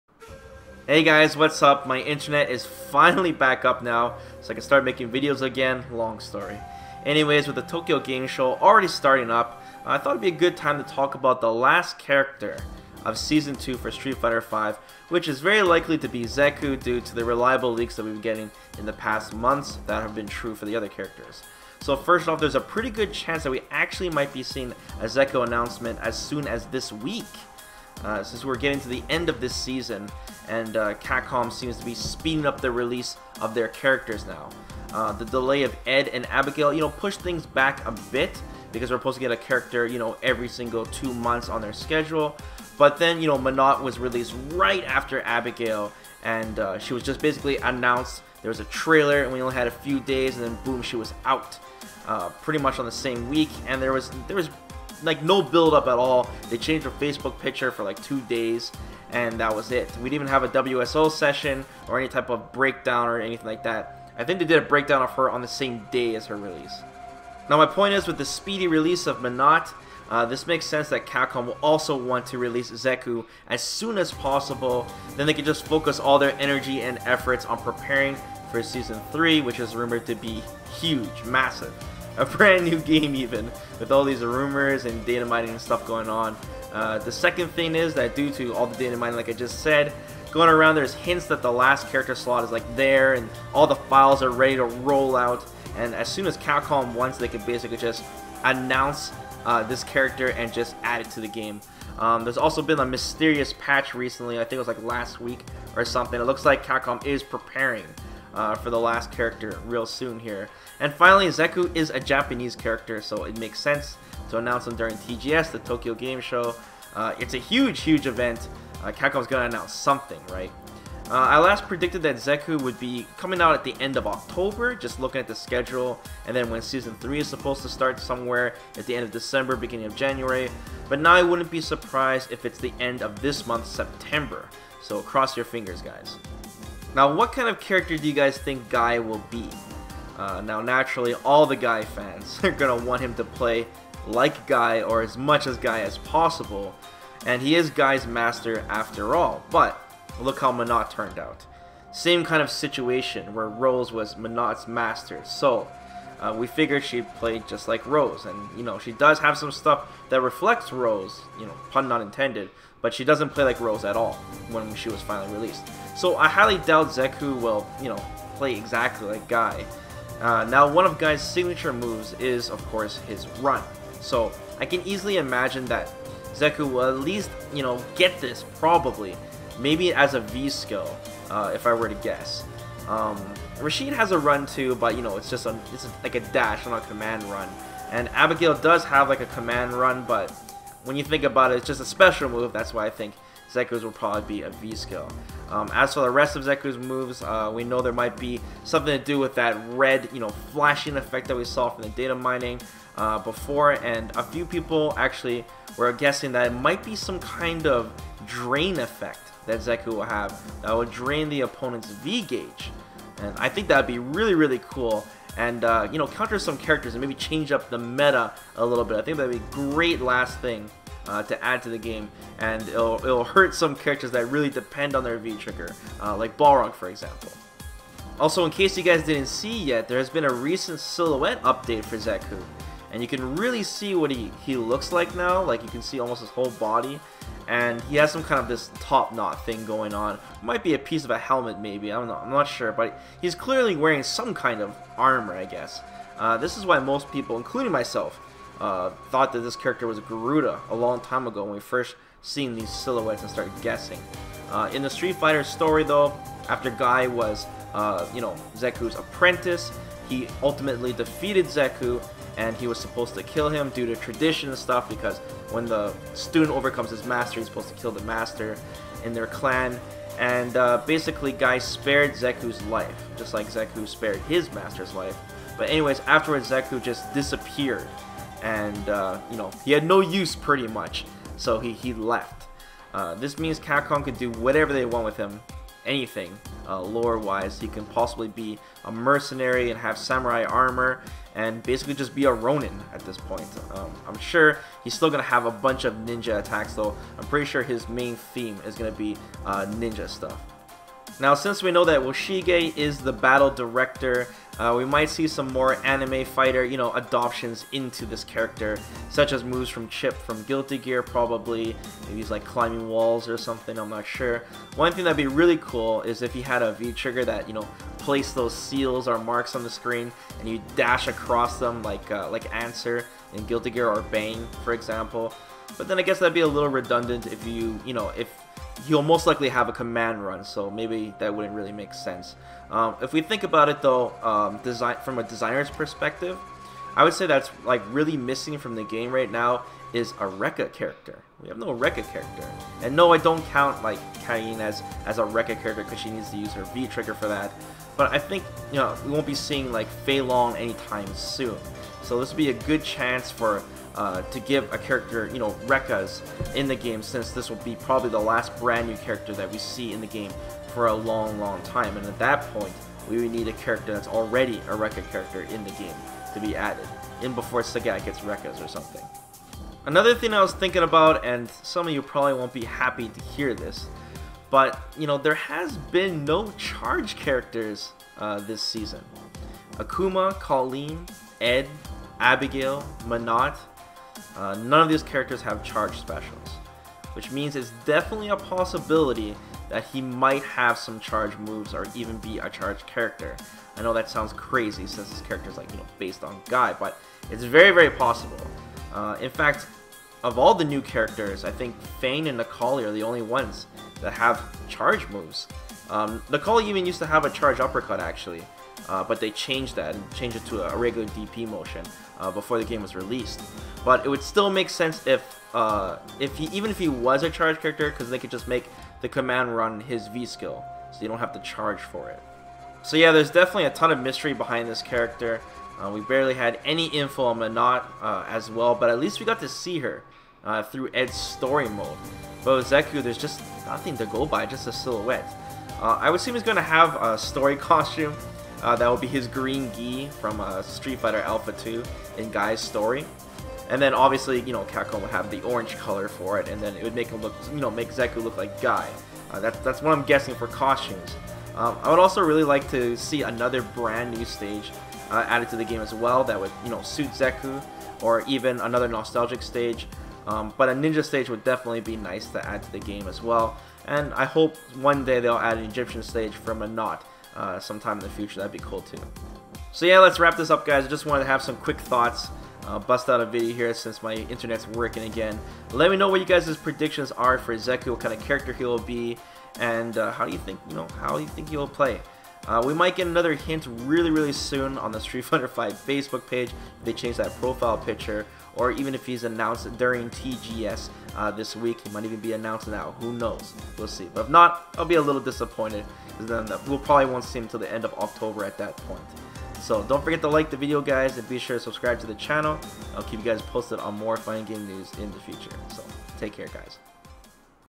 SFV - Zeku Coming Soon? Quick Thoughts On The Final New Character For Street Fighter 5
Street Fighter V has one last new character set to release for season 2. Due to all the leaks and rumors it is most likely going to be Zeku, Guy's master. The release might be very coming very soon since Menat came right after Abigail and TGS is taking place this week. I give my thoughts on how he will play for SF5.

Support VesperArcade!